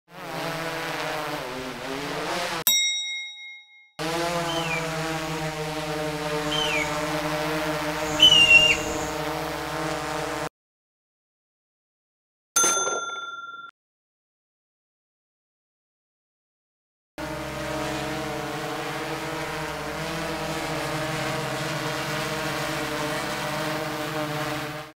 Respect the "home of the Fighting Hawks"
I was getting some footage of UND Football practice when a hawk decided to check my drone